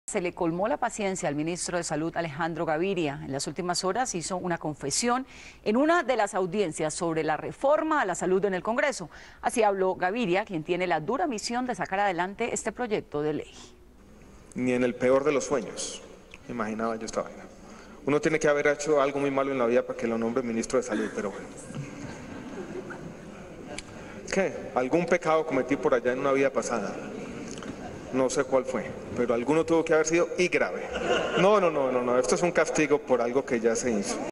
Ni en el peor de los sueños imaginaba esta vaina: Alejandro Gaviria - 23 de abril de 2013
El ministro, llevado por la desesperación con la Reforma a la Salud, aseguró que estar al frente de esta cartera es un castigo por algún pecado que cometió. - 23 de abril de 2013 Visita para mayor información